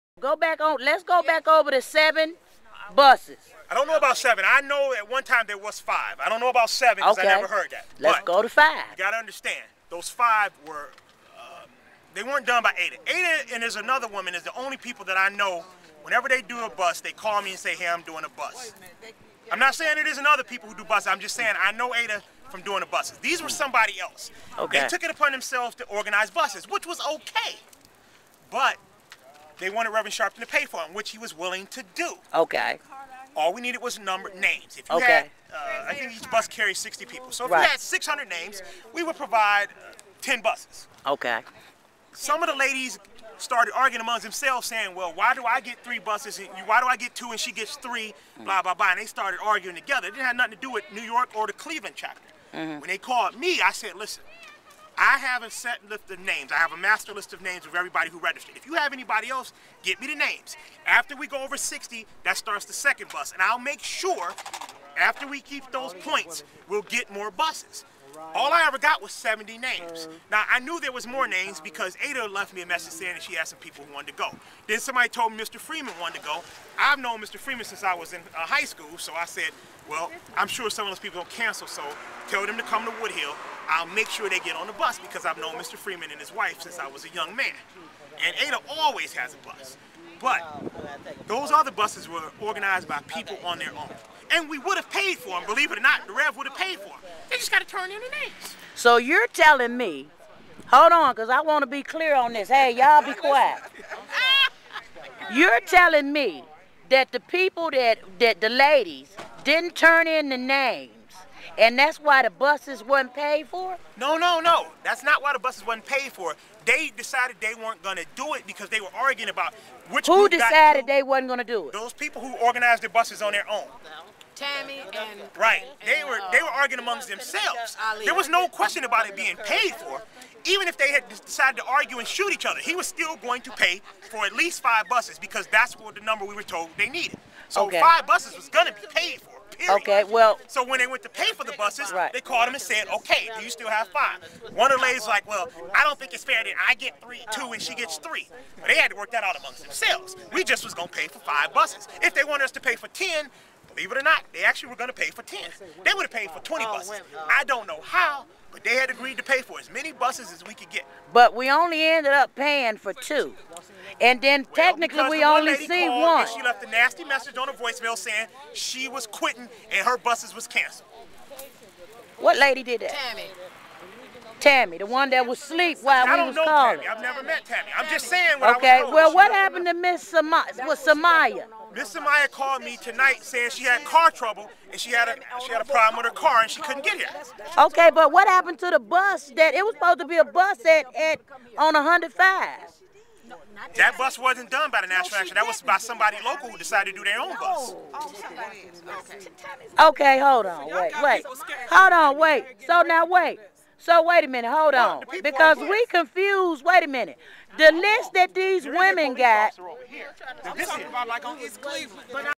CROOKED NATIONAL EDUCATION EQUALITY PROJECT(REV. AL SHARPTON)2009
RICHARD JONES -NATIONAL REPRESENTATIVE FOR NAN --IN CLEVELAND STARTS LIEING AT 11:OO P.M. UNTIL 1:30 P.M. ABOUT THE BUSES "NEVER SHOWING - UP"!PART11